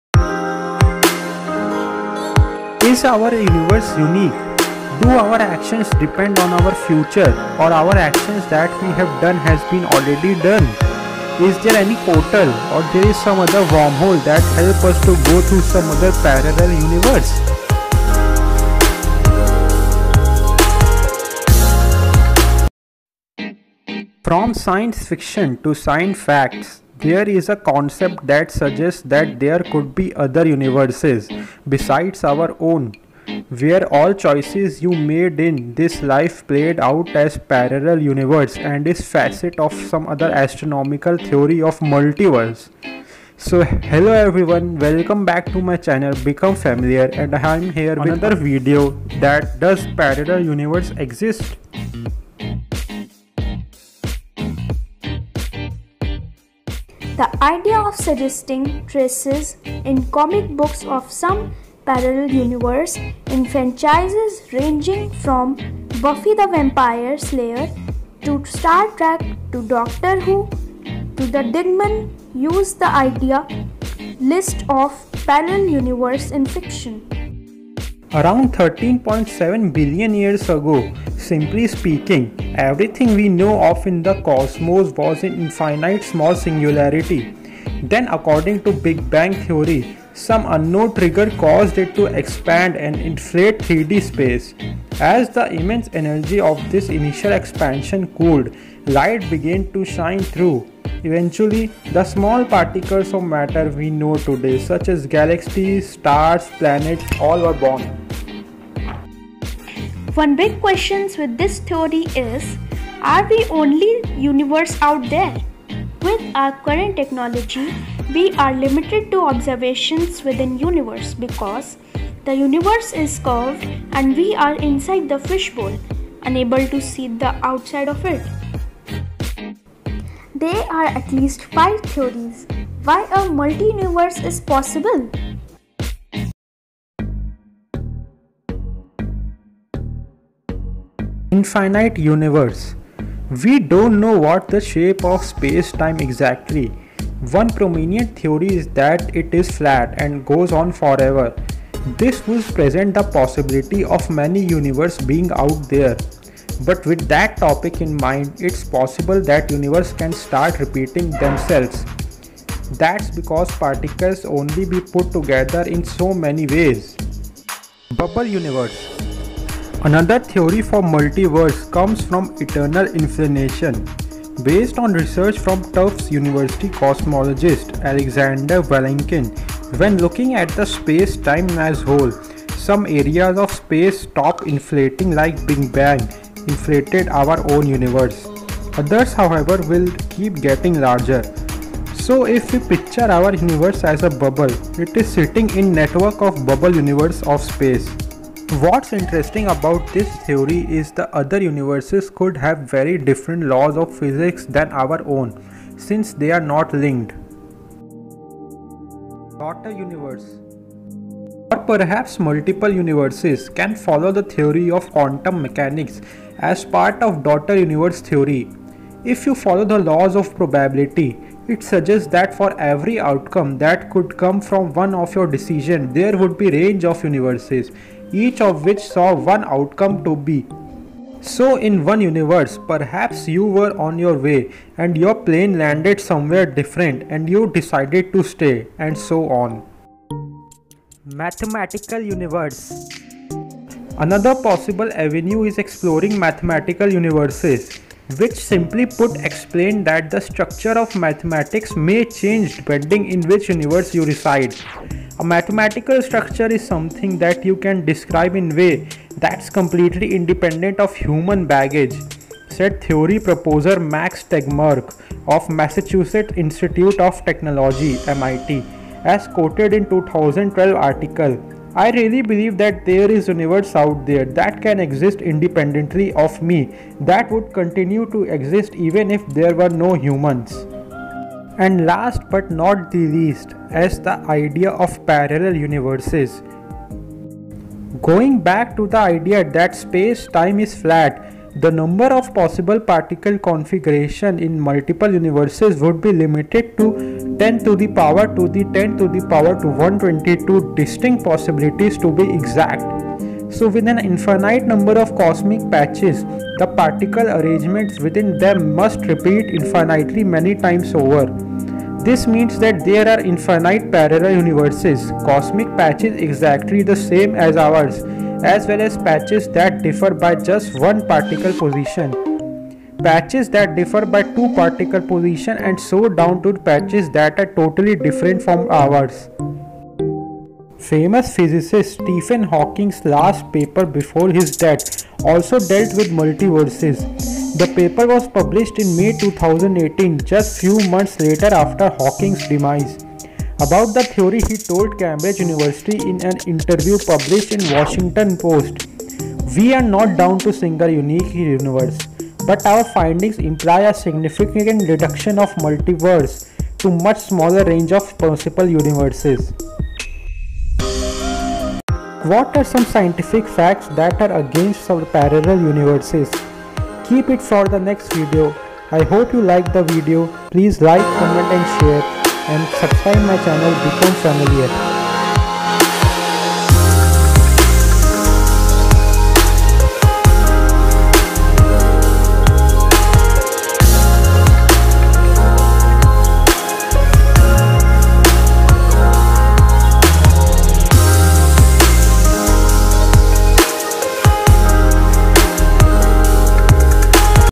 Does Parallel Universe exist: Theories and Facts
Does Parallel Universe exist?? What are theories and facts that prove of a multiverse.